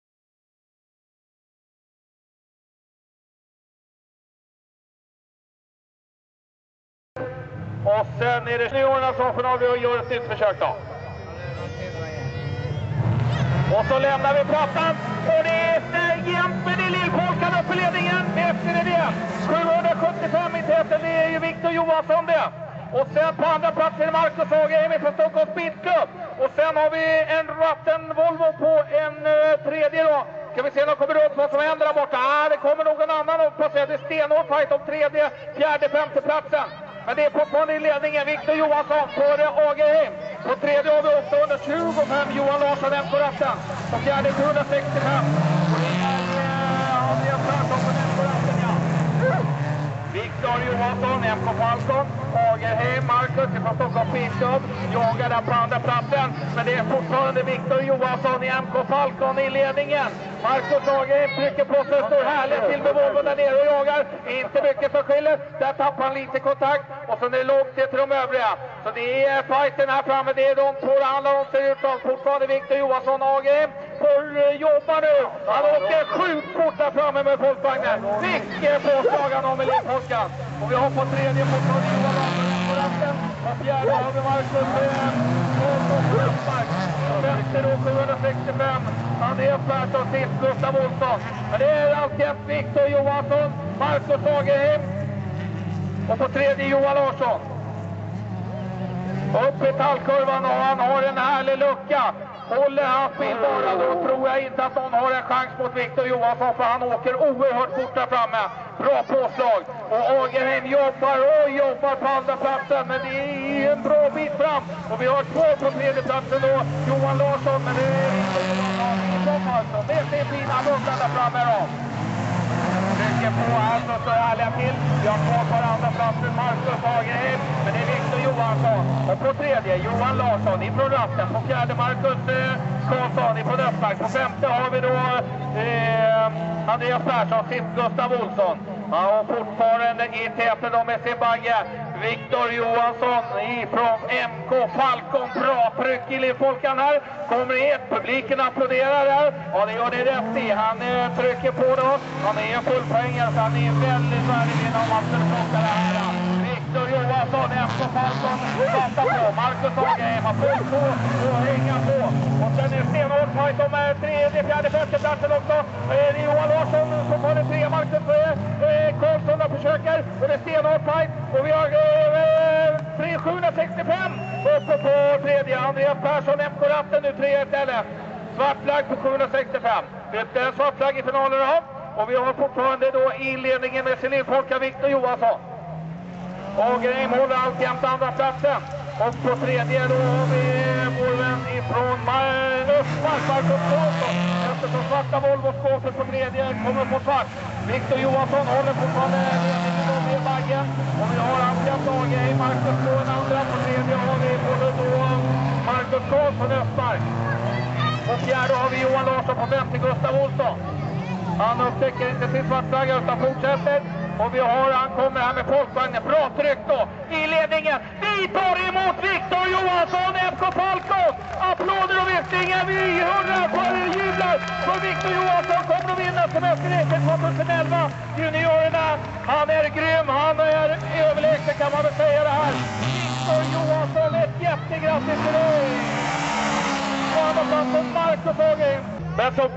A final juniorer Semesterracet 2011
Vinns enkelt av Victor Johansson